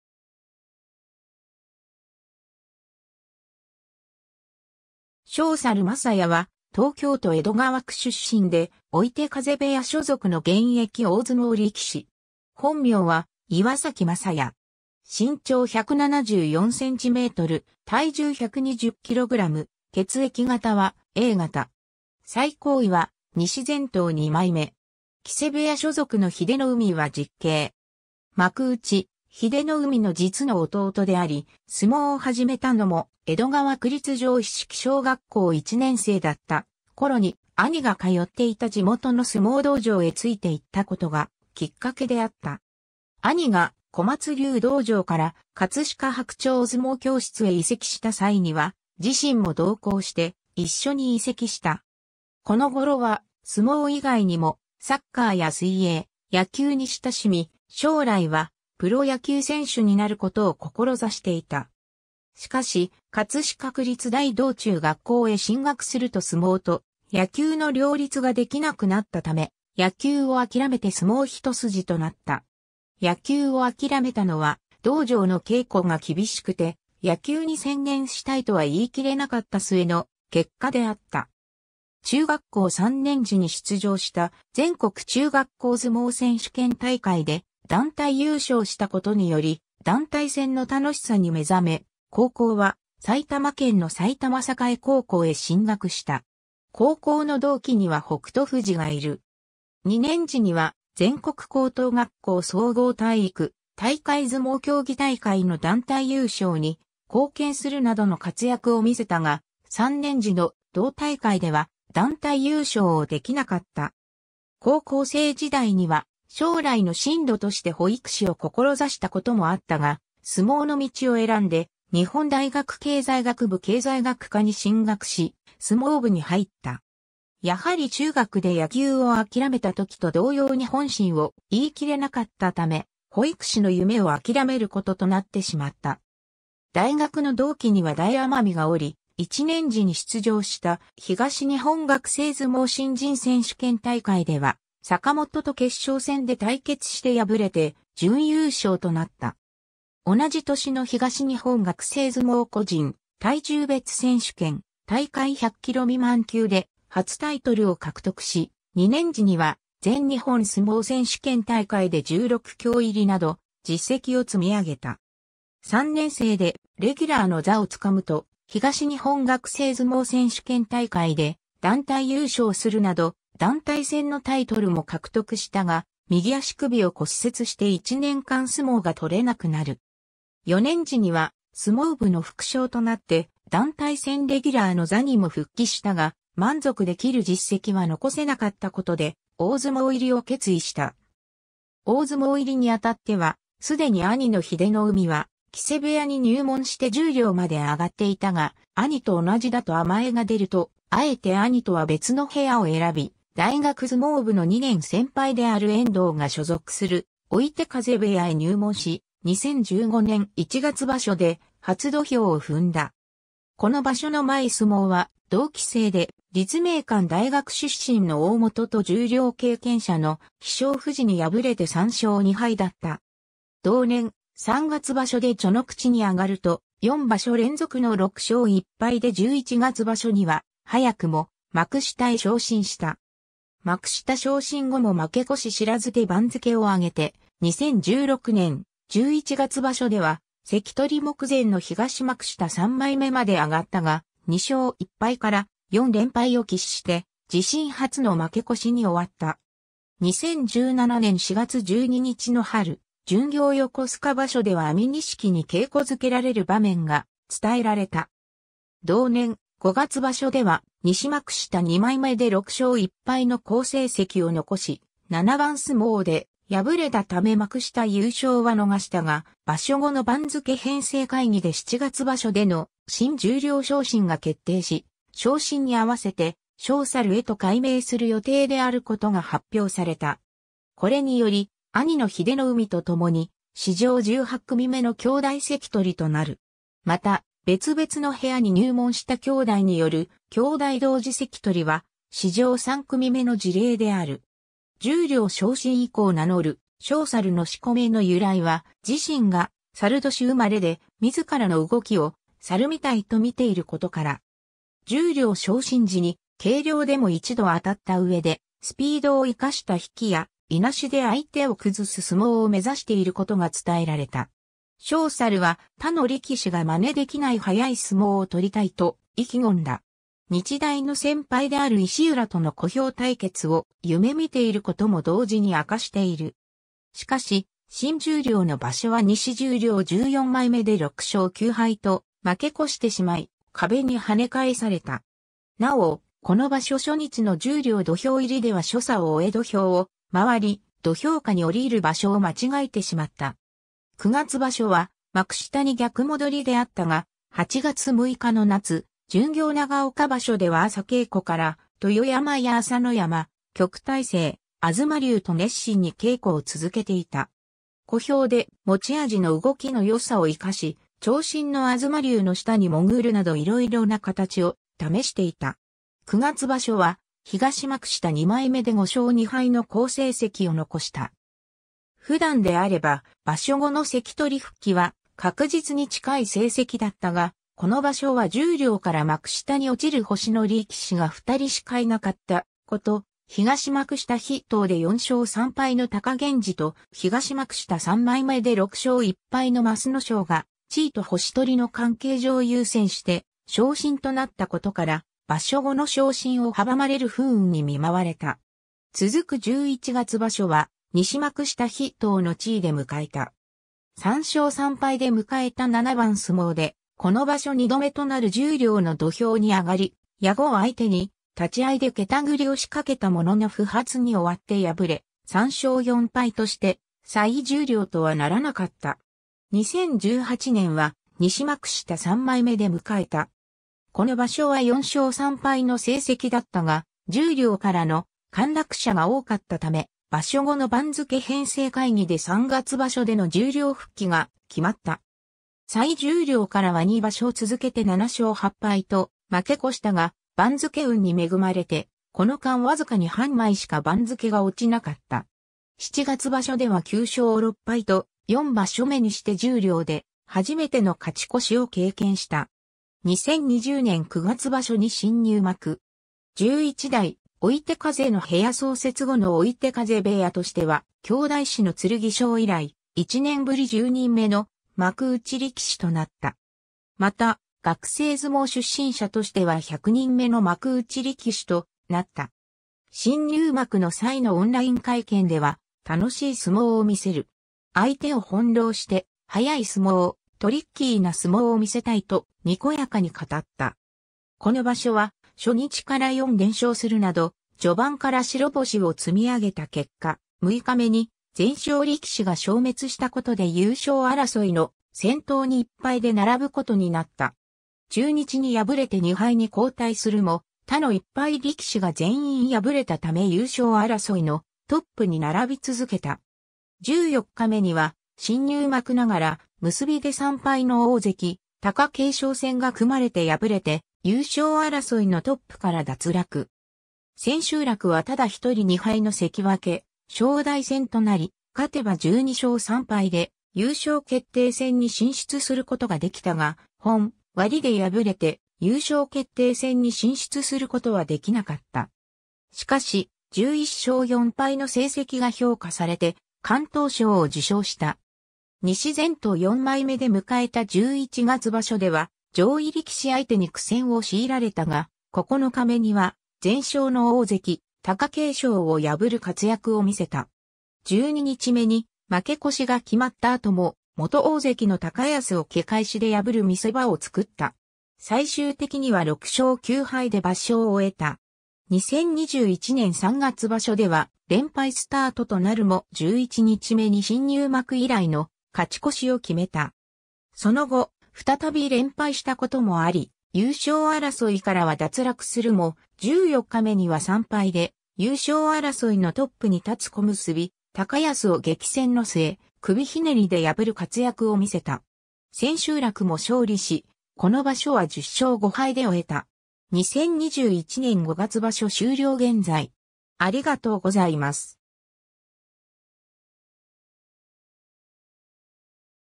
翔猿正也
翔猿 正也（とびざる まさや、1992年4月24日 - ）は、東京都江戸川区出身で、追手風部屋所属の現役大相撲力士。
本名は岩﨑 正也（いわさき まさや）。
身長174cm、体重120kg、血液型はA型。
最高位は西前頭2枚目（2021年5月場所）。
木瀬部屋所属の英乃海は実兄。
 幕内・英乃海（本名：岩﨑 拓也）の実の弟であり、相撲を始めたのも、江戸川区立上一色小学校1年生だった頃に兄が通っていた地元の相撲道場（小松竜道場）へついて行ったことがきっかけであった。
兄が小松竜道場から葛飾白鳥相撲教室へ移籍した際には、自身も同行して一緒に移籍した。
この頃は相撲以外にもサッカーや水泳、野球に親しみ、将来はプロ野球選手になることを志していた。
しかし、葛飾区立大道中学校へ進学すると相撲と野球の両立ができなくなったため、野球を諦めて相撲一筋となった。
野球を諦めたのは、道場の稽古が厳しくて「野球に専念したい」とは言い切れなかった末の結果であった。
中学校3年次に出場した全国中学校相撲選手権大会で団体優勝したことにより団体戦の楽しさに目覚め、高校は埼玉県の埼玉栄高校へ進学した。
高校の同期には北勝富士がいる。
2年次には全国高等学校総合体育大会相撲競技大会の団体優勝に貢献するなどの活躍を見せたが、3年次の同大会では団体優勝をできなかった。
高校生時代には将来の進路として保育士を志したこともあったが、相撲の道を選んで日本大学経済学部経済学科に進学し相撲部に入った。
やはり中学で野球を諦めた時と同様に本心を言い切れなかったため、保育士の夢を諦めることとなってしまった。
大学の同期には大奄美（本名：坂元 元規）がおり、1年次に出場した東日本学生相撲新人選手権大会では、坂元と決勝戦で対決して敗れて準優勝となった。
同じ年の東日本学生相撲個人体重別選手権大会100キロ未満級で初タイトルを獲得し、2年次には全日本相撲選手権大会で16強入りなど、実績を積み上げた。
3年生でレギュラーの座を掴むと東日本学生相撲選手権大会で団体優勝するなど、団体戦のタイトルも獲得したが、右足首を骨折して1年間相撲が取れなくなる。
4年次には相撲部の副将となって団体戦レギュラーの座にも復帰したが、満足できる実績は残せなかったことで、大相撲入りを決意した。
 大相撲入りにあたっては、既に兄の英乃海は木瀬部屋に入門して十両まで上がっていたが、「兄と同じだと甘えが出る」と敢えて兄とは別の部屋を選び、大学相撲部の2年先輩である遠藤が所属する追手風部屋（元前頭・大翔山）へ入門し、2015年1月場所で初土俵を踏んだ。
この場所の前相撲は、同期生で立命館大学出身の大元と十両経験者の飛翔富士に敗れて3勝2敗だった。
同年3月場所で序ノ口に上がると、4場所連続の6勝1敗で11月場所には早くも幕下へ昇進した。
幕下昇進後も負け越し知らずで番付を上げて、2016年11月場所では関取目前の東幕下3枚目まで上がったが、2勝1敗から4連敗を喫して自身初の負け越しに終わった。
2017年4月12日の春巡業横須賀場所では安美錦に稽古づけられる場面が伝えられた。
同年5月場所では西幕下2枚目で6勝1敗の好成績を残し、7番相撲で敗れたため幕下優勝は逃したが、場所後の番付編成会議で7月場所での新十両昇進が決定し、昇進に合わせて「翔猿」へと改名する予定であることが発表された。
これにより、兄の英乃海とともに史上18組目の兄弟関取となる。
また、別々の部屋に入門した兄弟による兄弟同時関取は、史上3組目の事例である。
十両昇進以降名乗る「翔猿」の四股名の由来は自身が申年生まれで、自らの動きを「猿みたい」とみていることから。
十両昇進時に、軽量でも一度当たった上で、スピードを生かした引きや、いなしで相手を崩す相撲を目指していることが伝えられた。
翔猿は「他の力士がまねできない速い相撲を取りたい」と意気込んだ。
日大の先輩である石浦との小兵対決を夢見ていることも同時に明かしている。
しかし、新十両の場所は西十両14枚目で6勝9敗と負け越してしまい、壁に跳ね返された。
なお、この場所初日の十両土俵入りでは所作を終え土俵を回り、土俵下に下りる場所を間違えてしまった。
9月場所は幕下に逆戻りであったが、8月6日の夏巡業長岡場所では朝稽古から豊山や朝乃山、旭大星、東龍と熱心に稽古を続けていた。
小兵で持ち味の動きの良さ...